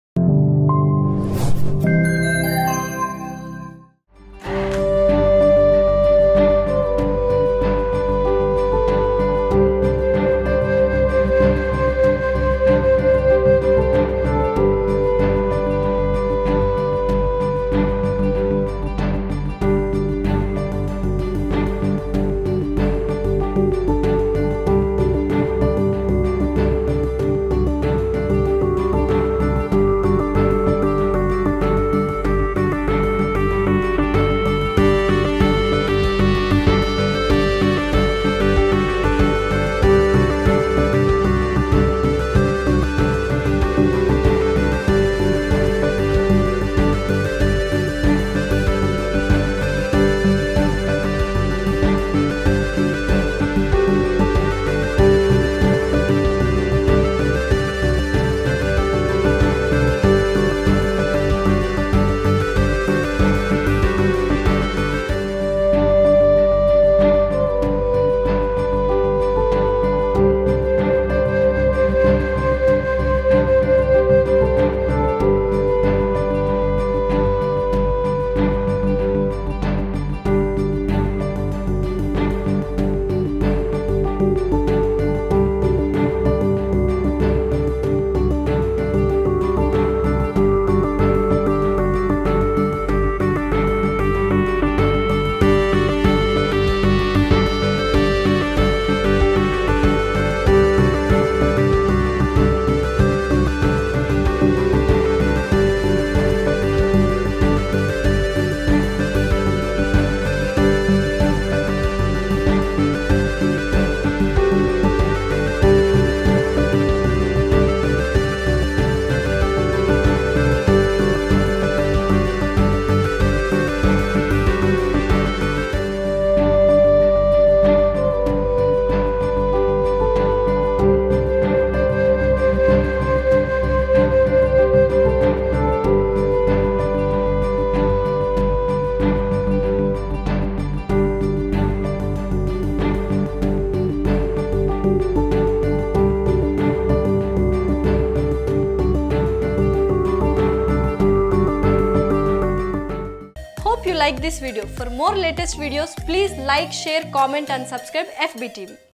Heart Touching Video | FB TV | Asvi Media
► About The Video :Heart Touching Video: Friends & Well Wishers Support To Woman During Lockdown | FB TV | Asvi Media |